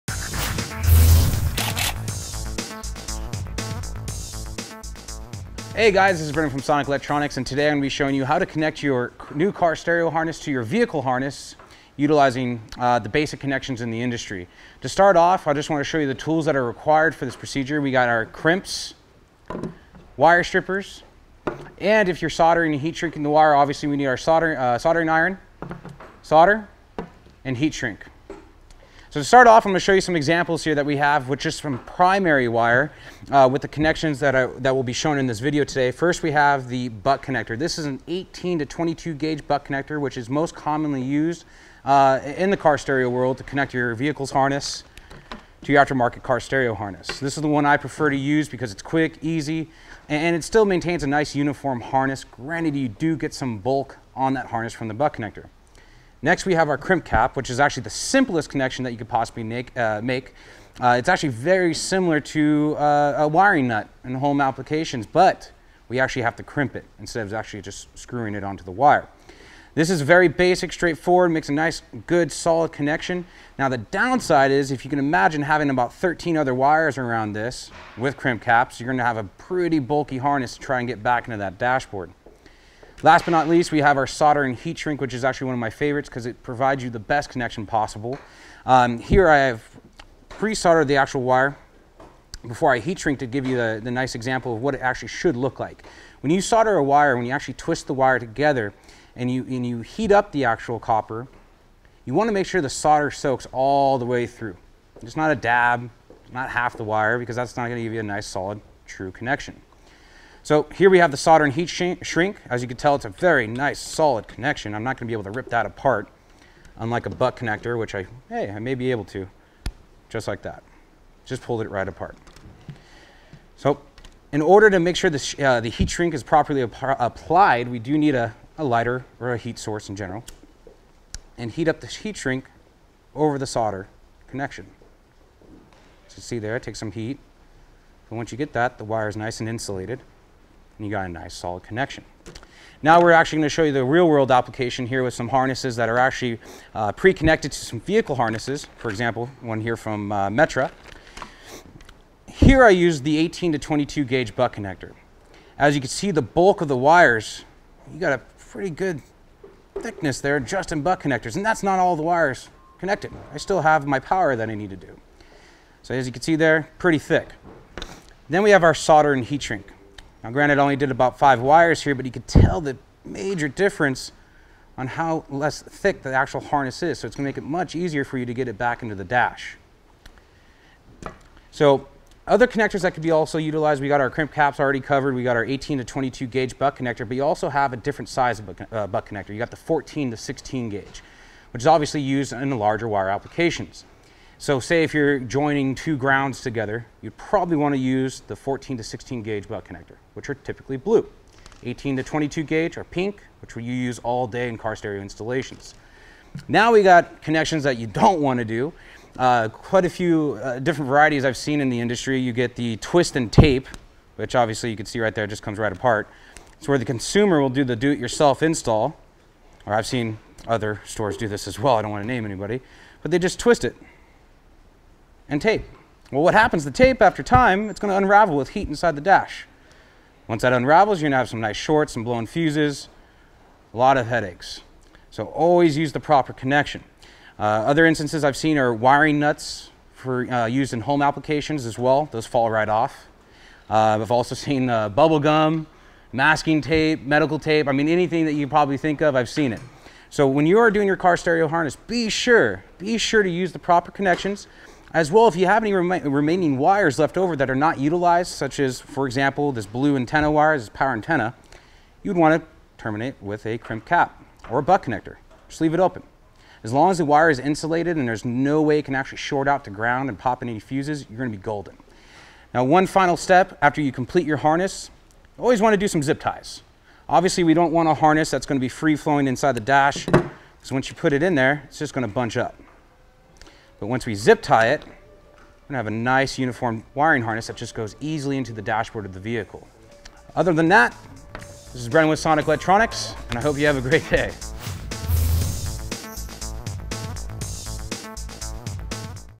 How to Properly Connect A Wire Harness | Car Audio 101
Learn about the different methods of connecting the wires on your wiring harness. The three ways we demonstrate in this video are the following:
1) Butt Connectors
2) Crimp Caps
3) Solder & Heat Shrink

Overall, we recommend soldering all wires because it created a secure, solid connection that isn't going to short out over time. However, the down side to soldering is that it takes a lot longer.
Butt connectors are the most popular and widely used connector because of how easy they are to install. Crimp caps are fast and secure, but they are bulky and take up a lot of space.

To buy your installation accessories, be sure to visit Sonic Electronix for all your installation needs.